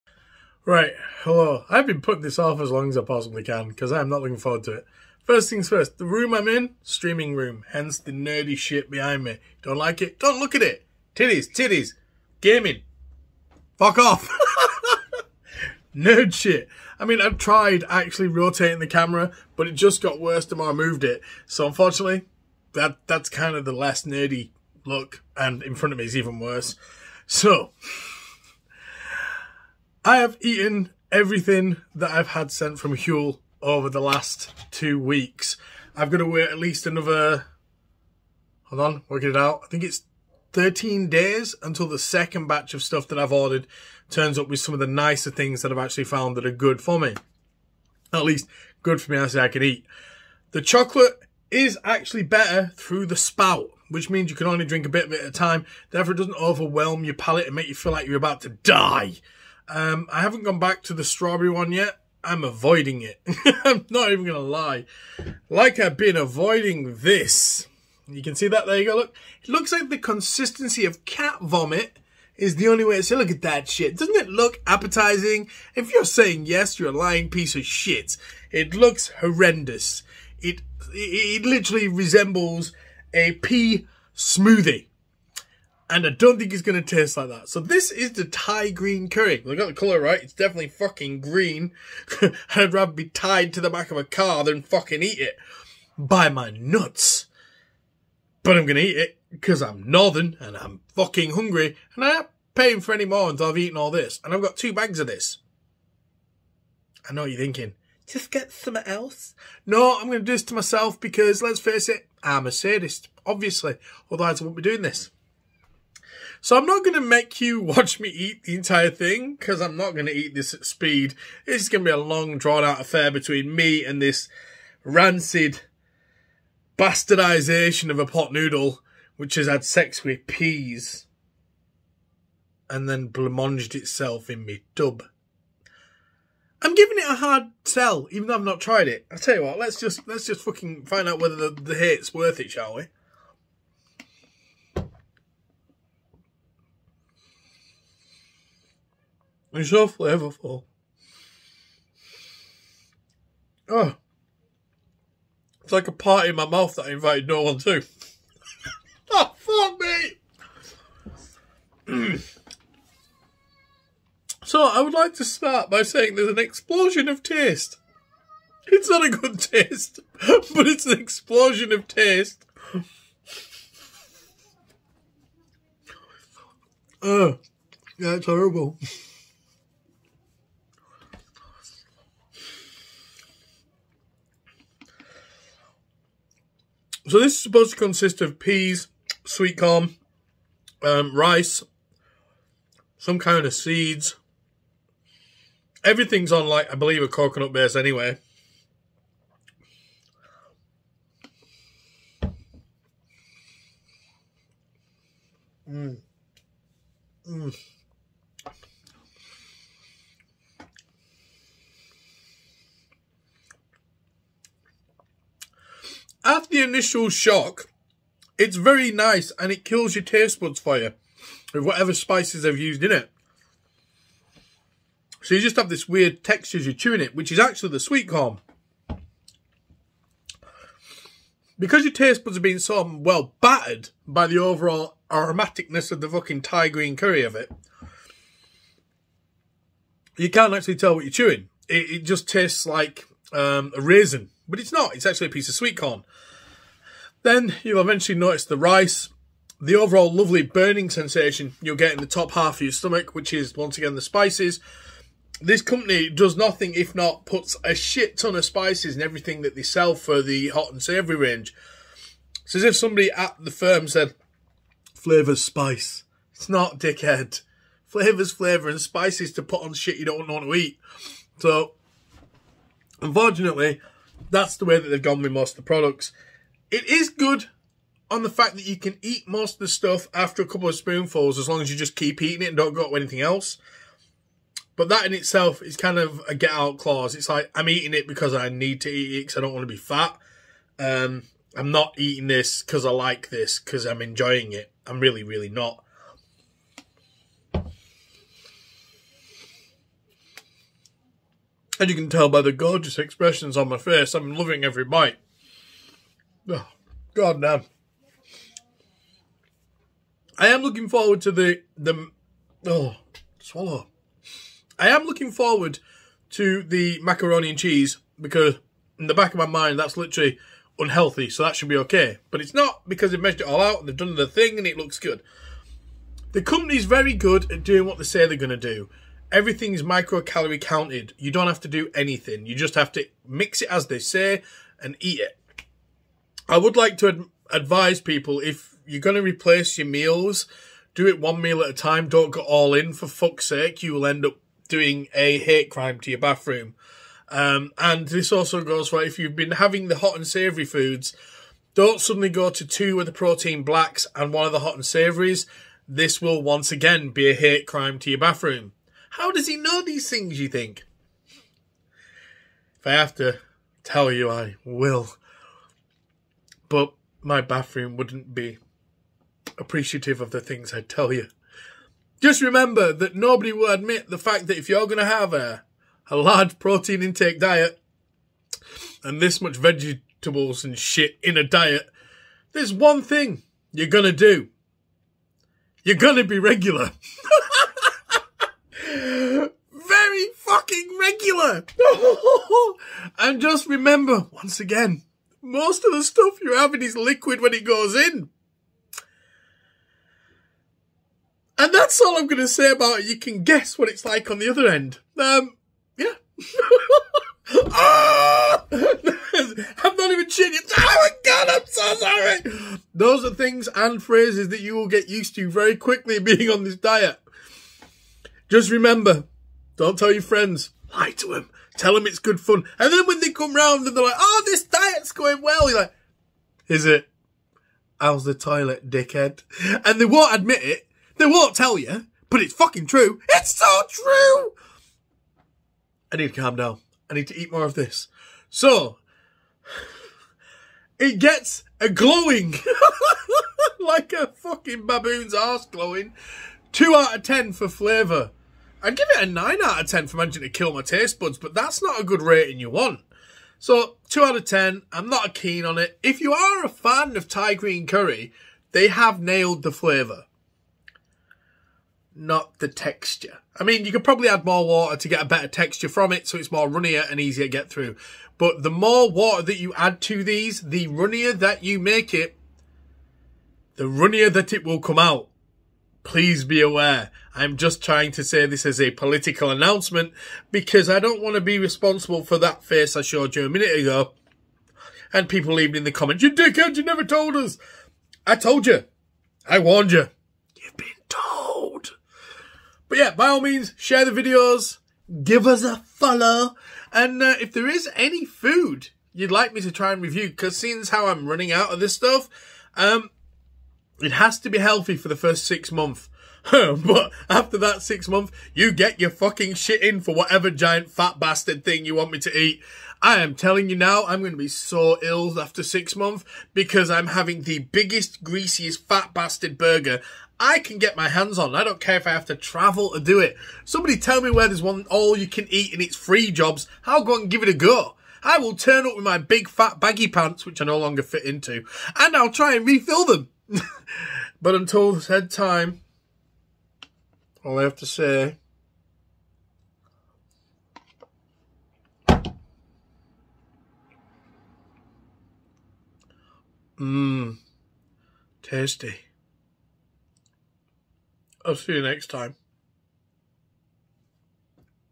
huel review tai Green curry
honest review just me and the food I'm eating to slim down